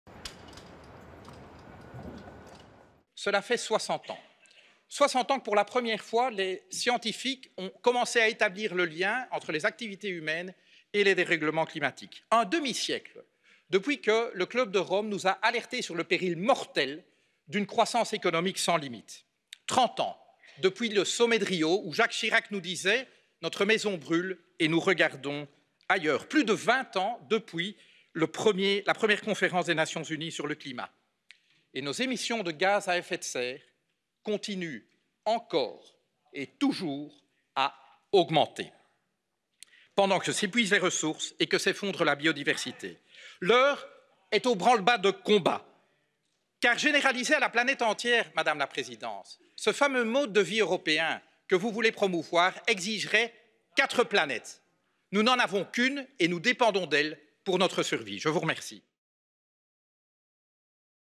Philippe Lamberts on the European Green Deal
The Commission's European Green Deal is a necessary step but now we need to see real climate action! Greens/EFA Co-President Philippe Lamberts sets out what we think the deal should achieve:

We hope it works to:

- help limit global warming to 1.5C
- reduce social inequalities
- replace neo-liberal model with a sustainable model
- put finance at service of people